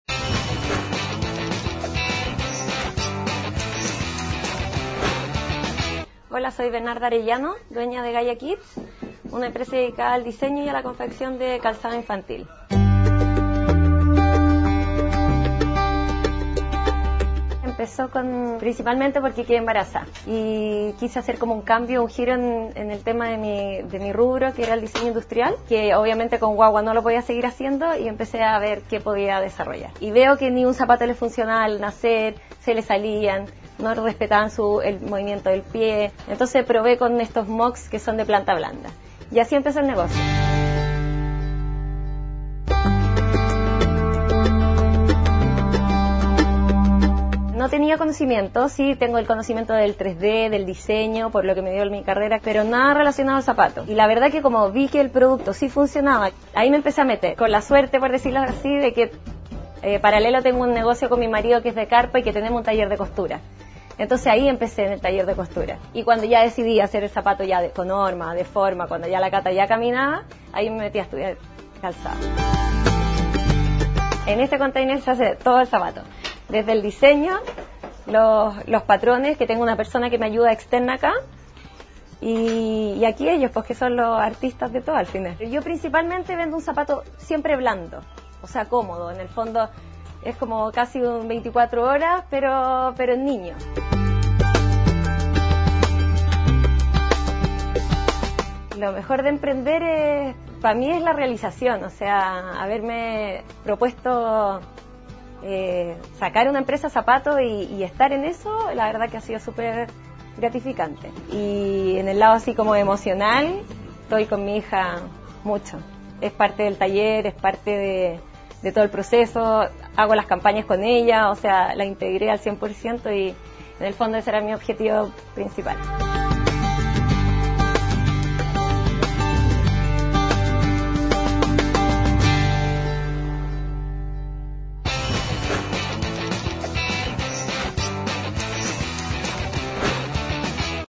Hecho en Chile: Gaia Kids
La marca nació hace un año con el nacimiento de Catalina, la hija de Bernada Arellano.